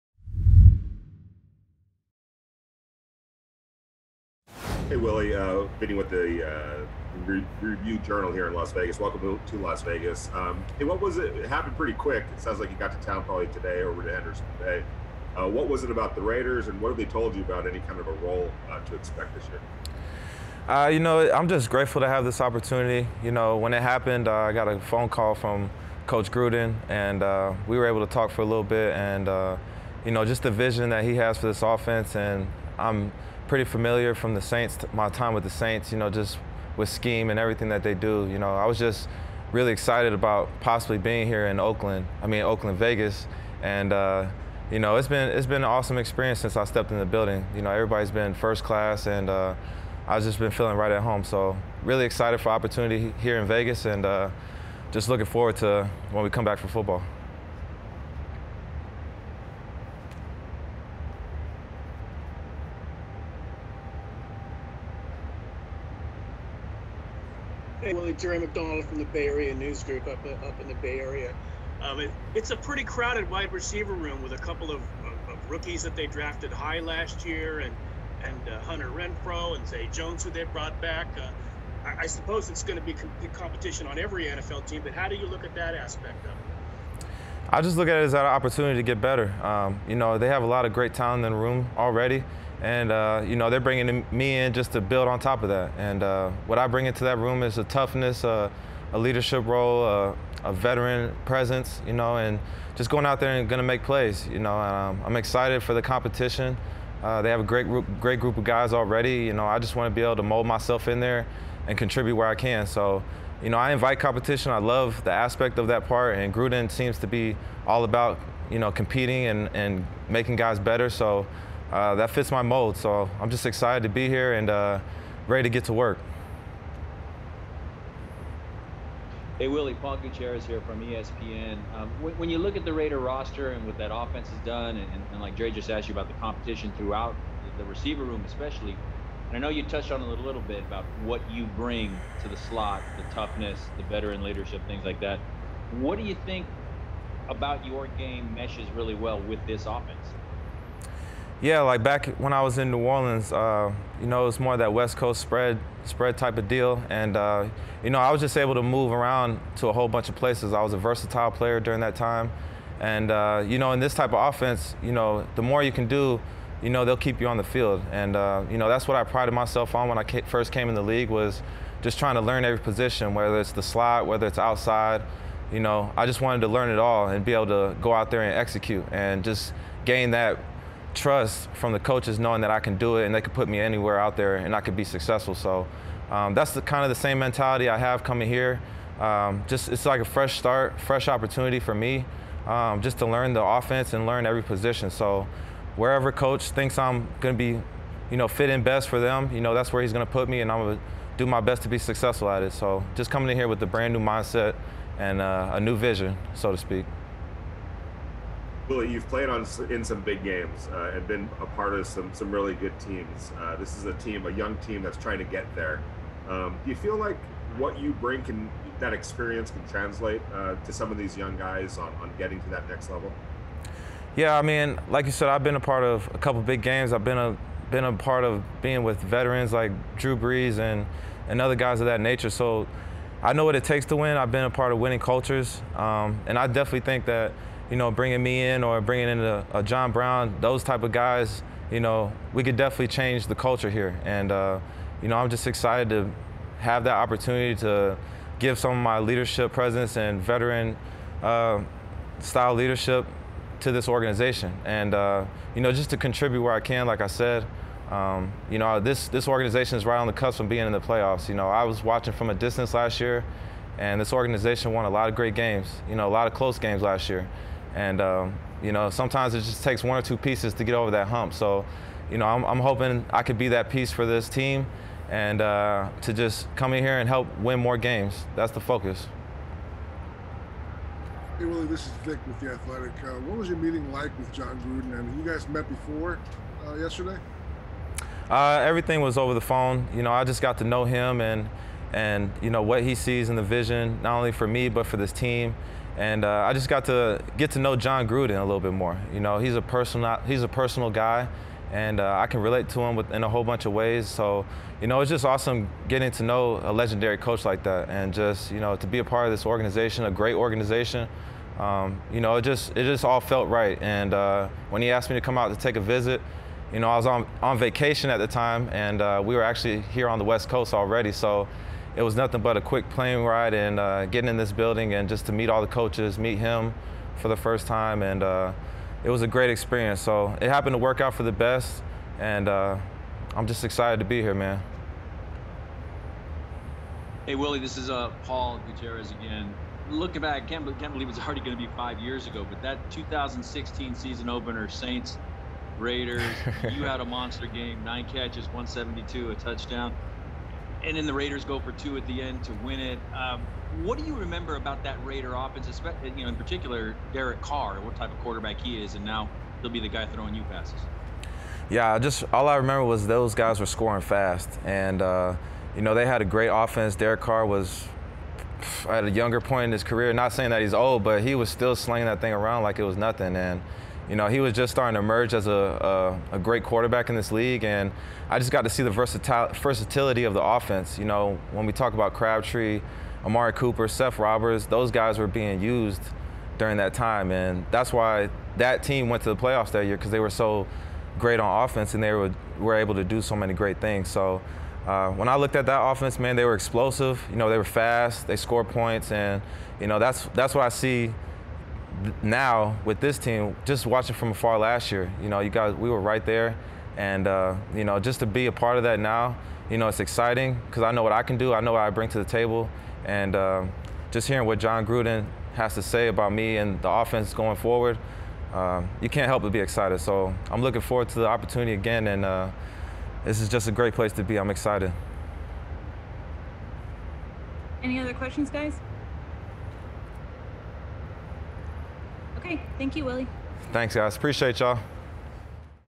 Willie Snead IV on 'New Vision' With Raiders, Competition at WR, Coach Gruden & More
WR Willie Snead IV discusses signing with the Raiders, his mindset for 2021, getting to know Head Coach Jon Gruden, QB Derek Carr, and more.